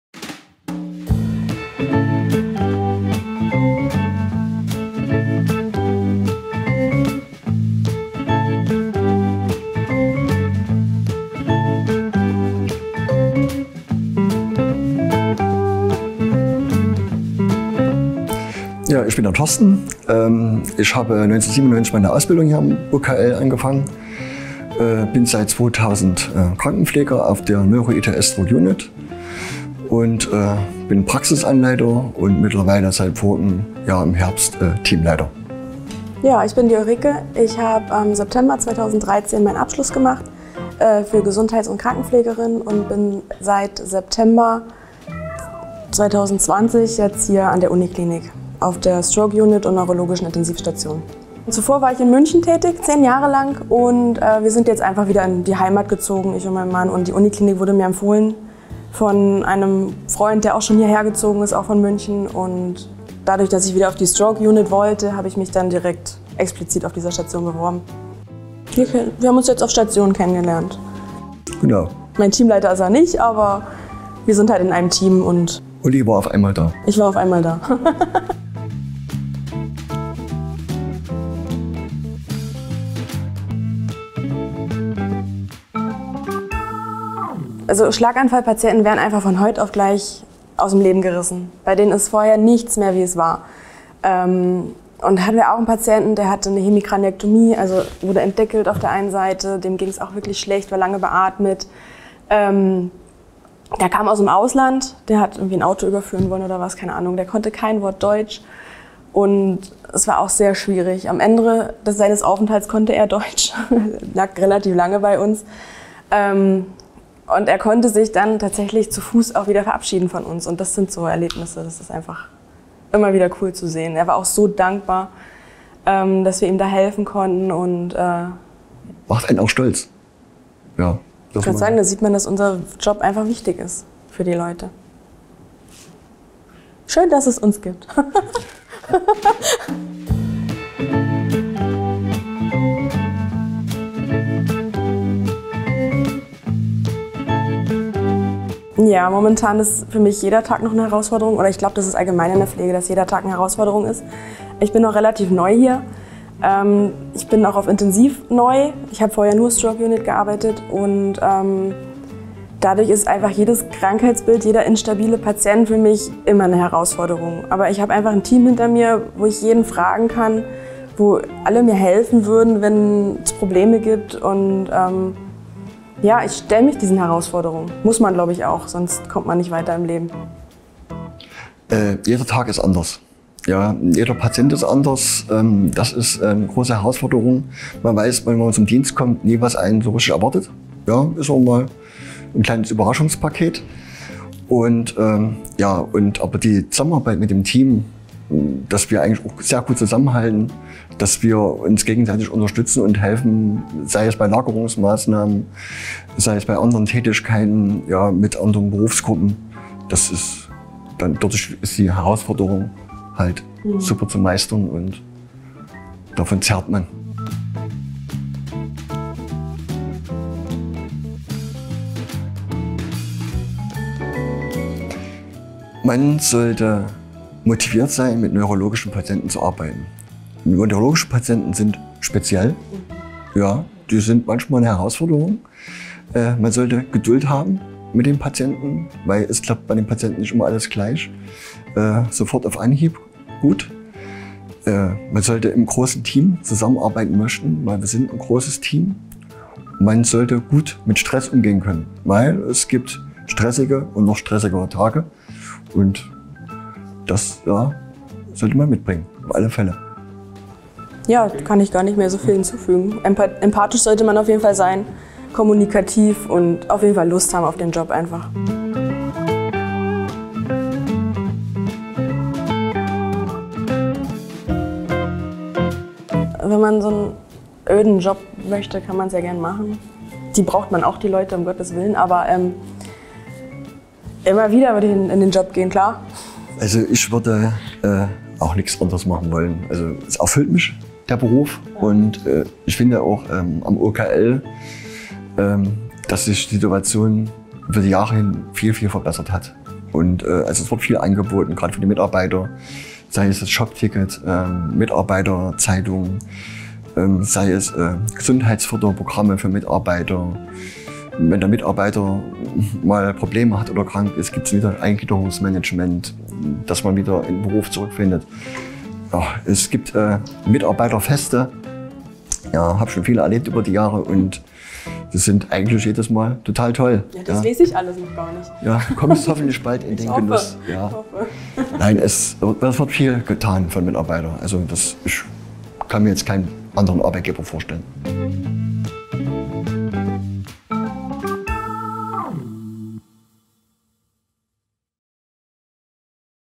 Pflege ist... | Ulrike Schneider & Torsten Reichel über ihre Arbeit auf der ITS/Stroke Unit G 1.1
„Wir sind stolz auf unsere Kolleginnen und Kollegen in der Pflege, die vor allem auch jetzt bei der Pandemiebewältigung großartige Arbeit geleistet haben und leisten“, sagt Prof. Christoph Josten, Medizinischer Vorstand am UKL,  “und wir wissen – wir  brauchen die Pflege wie der Mensch das Wasser“.  „Die vergangenen Monate haben allen vor Augen geführt, welche große Bedeutung den in den Pflegeberufen Tätigen in der Medizin zukommt“, ergänzt der Kaufmännische Vorstand Dr. Robert Jacob.

Um Euch einen Einblick in die großartige Arbeit unserer Pflegenden zu geben, hier der zweite Teil unserer Serie "Pflege ist...", in der Pflegende des UKL von ihren Erfahrungen und Motivationen erzählen.
Stellvertretend für das Pflegeteam der Intensivtherapiestation und Stroke Unit G 1.1  berichten Gesundheits- und Krankenpflegerin Ulrike Schneider sowie Krankenpfleger, Teamleiter und Praxisanleiter Torsten Reichel über ihren Alltag am UKL.